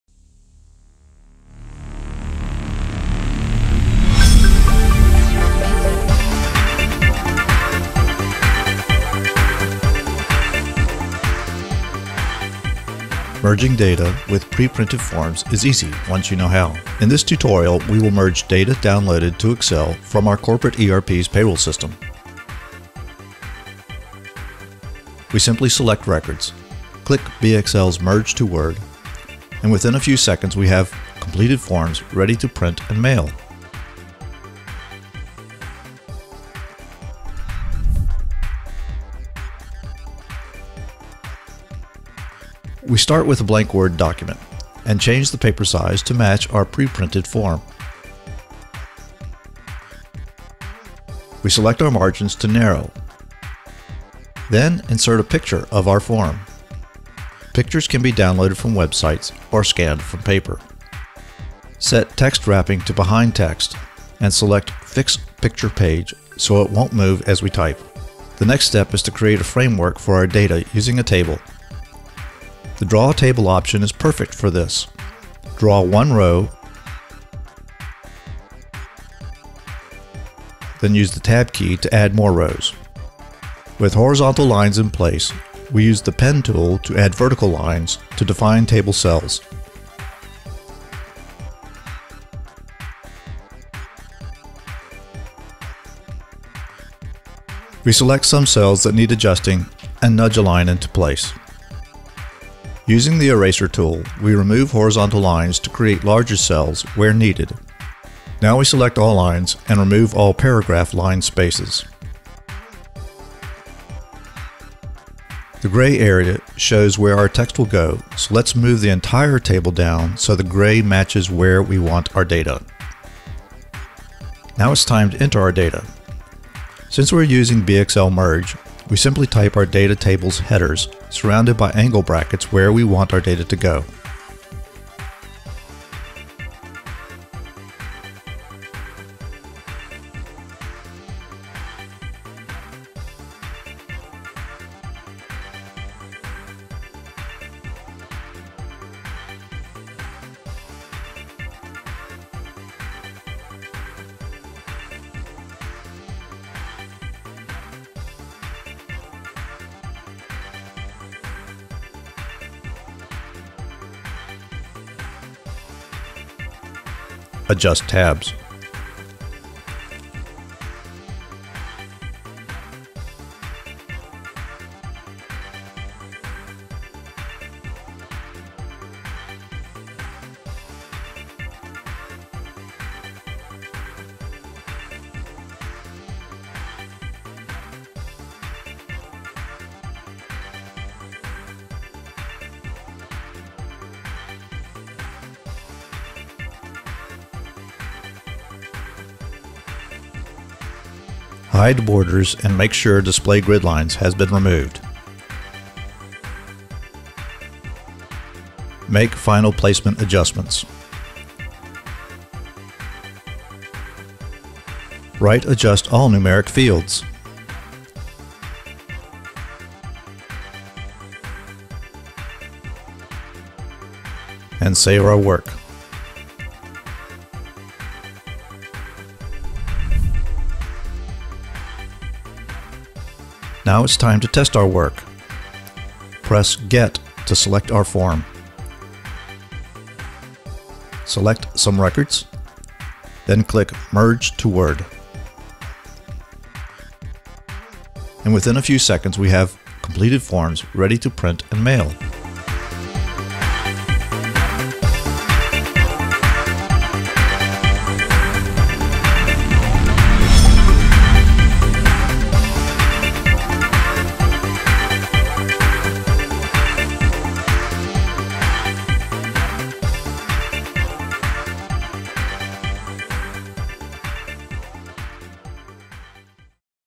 Create mail merge template from un-editable, preprinted form then merge w/Excel records.
Free Excel Add-in available at Eloquens.Com

Merging Excel data to preprinted, un-editable forms can seem impossible until you know how - then it's simple.

This 5 minute tutorial takes a W2 form downloaded from the IRS (US tax bureau) and shows how to prepare it for mail merge. The technique generally applies to MS Word's mail merge but in this video it is tailored to BXL's Merge (free) Add-in. This tutorial then shows how we can merge Excel data, simulating payroll records exported from just about any Payroll system or ERP, to the W2.

This technique works great for HR benefits and policy forms, Medical Forms, Insurance Forms, Government Forms, Legal forms, applicant and new hire onboarding forms, Contracts, invoices, and more.